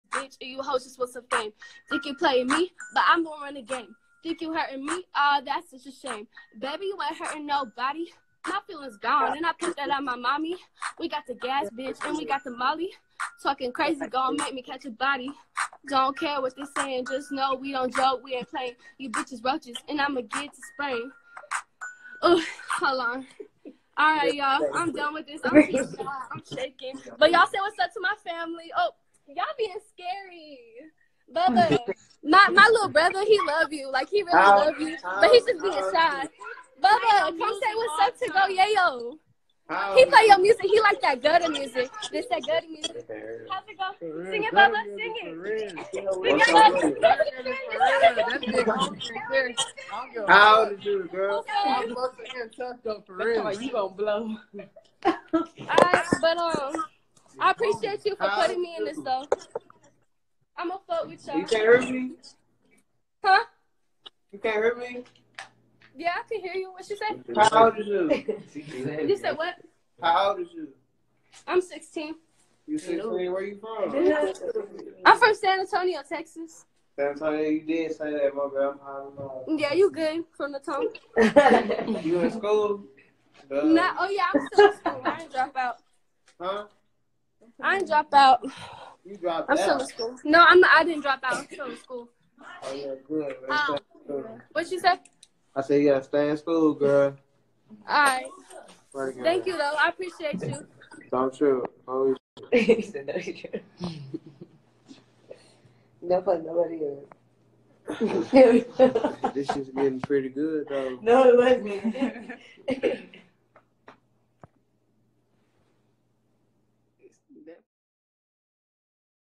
GO YAYO - Falls Asleep On Fan On Instagram Live While She Raps For Him
Go Yayo Falls Asleep 😴 On Fan While She’s Freestyling For Him on Instagram Live!